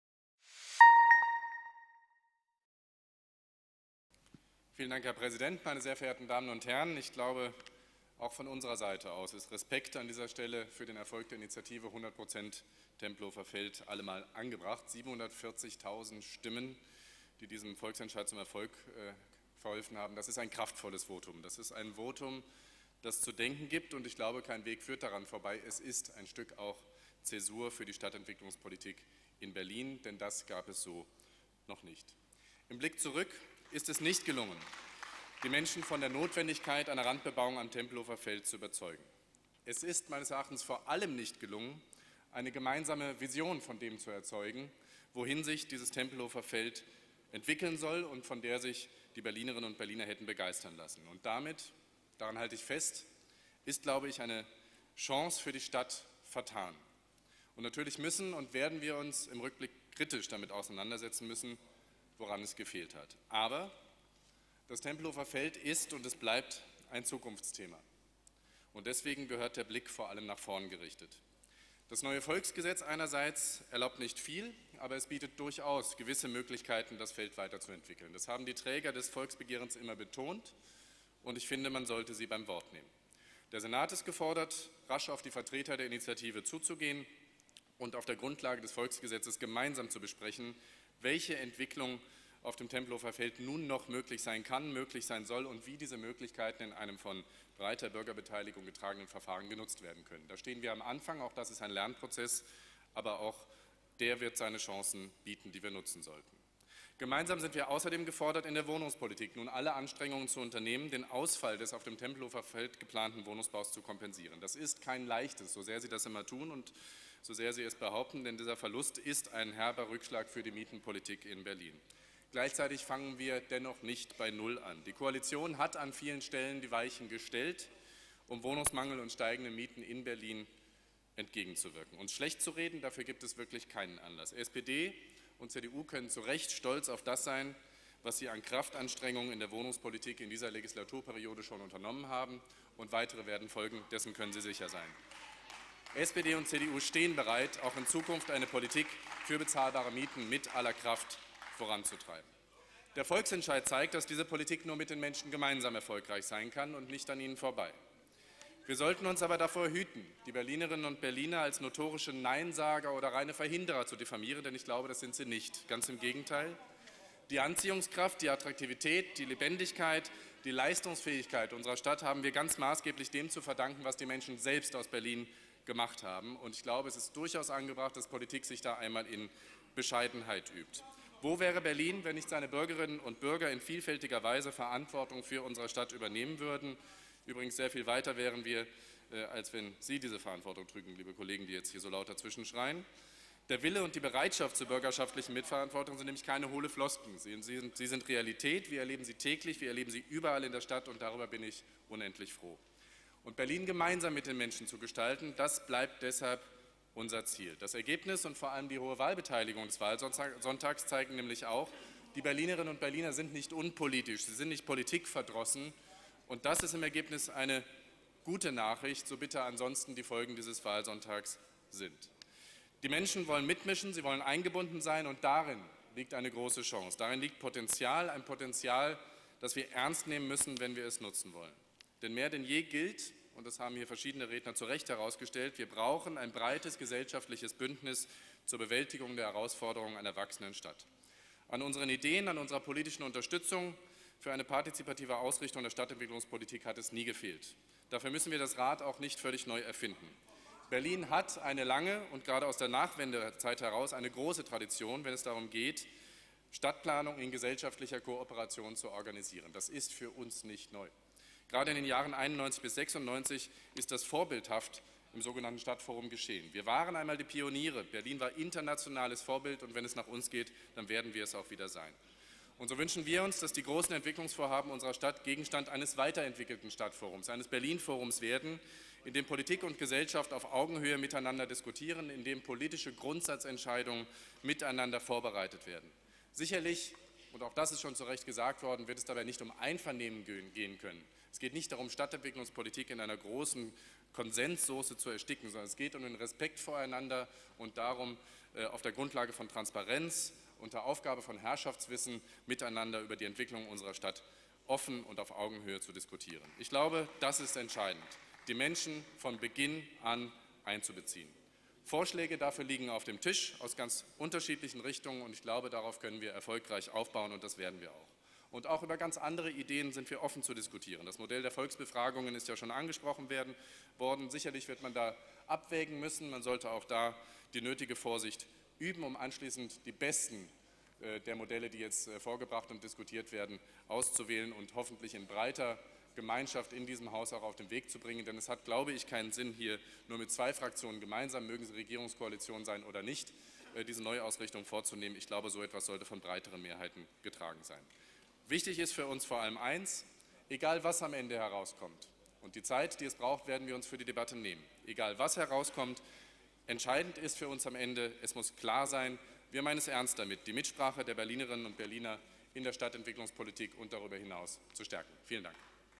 49. Sitzung des Berliner Abgeordnetenhauses - Tempelhofer Feld - Stefan Evers (CDU)
49. Sitzung des Berliner Abgeordnetenhauses - 05. Juni 2014
Ergebnisse des Tempelhof-Volksentscheids ernst nehmen: Der Weg ist frei
Stefan Evers (CDU)